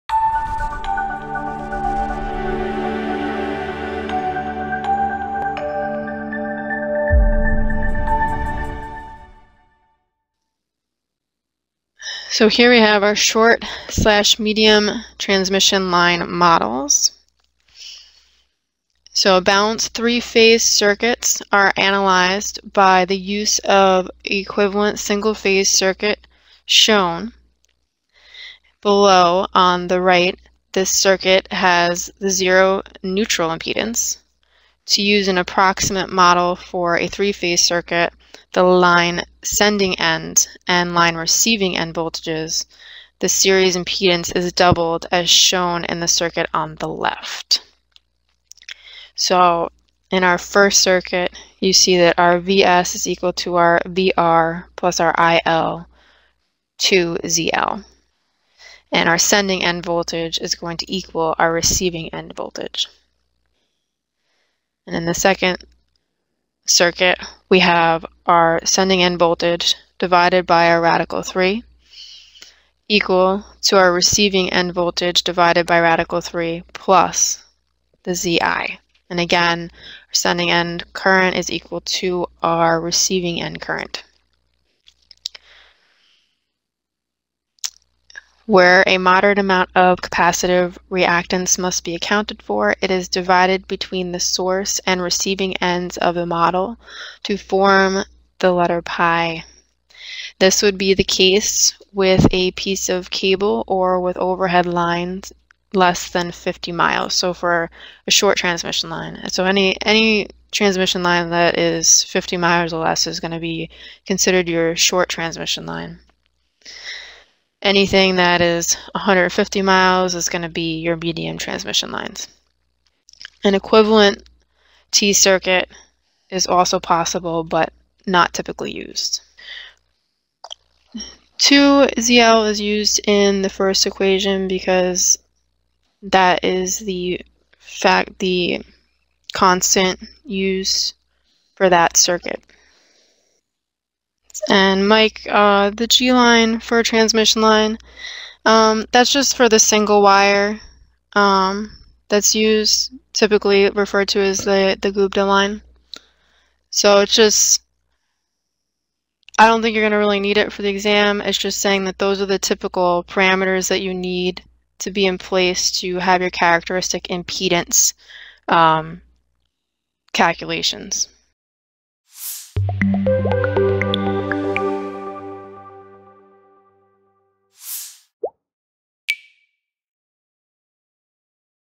What Is a Short Medium Transmission Line Model? | PE Electrical
Thinking about enrolling in a Live Online Review Course, but unsure what to expect?  Here is a short sample video from one of our classes.

Follow @SchoolofPe